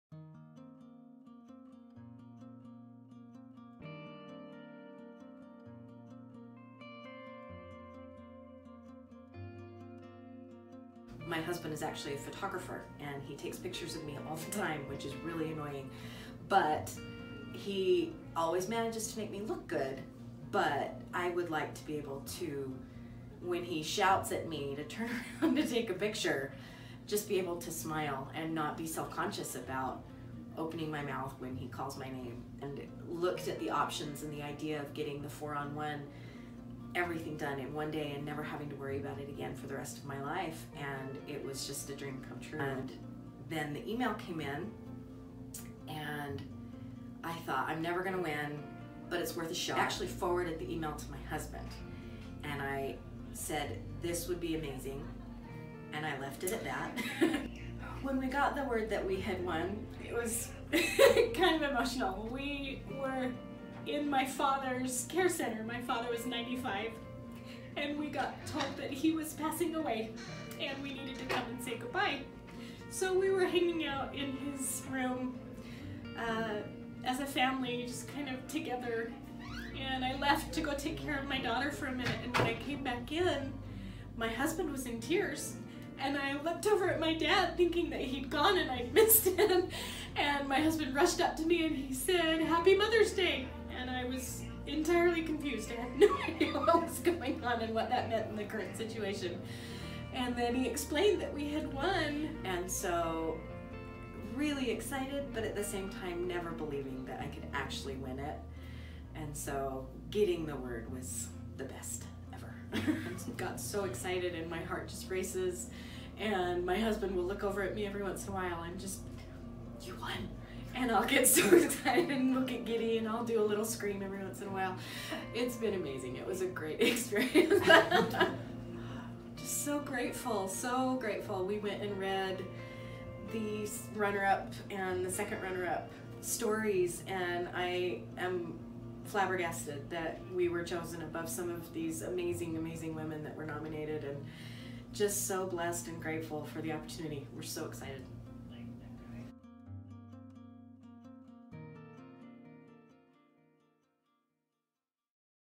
Mother's Day Smile Makeover Lori's Thoughts on Winning
Lori describes her feelings about winning our 2019 Mother's Day Complete Smile Makeover Giveaway. We can't wait to watch Lori's transformation.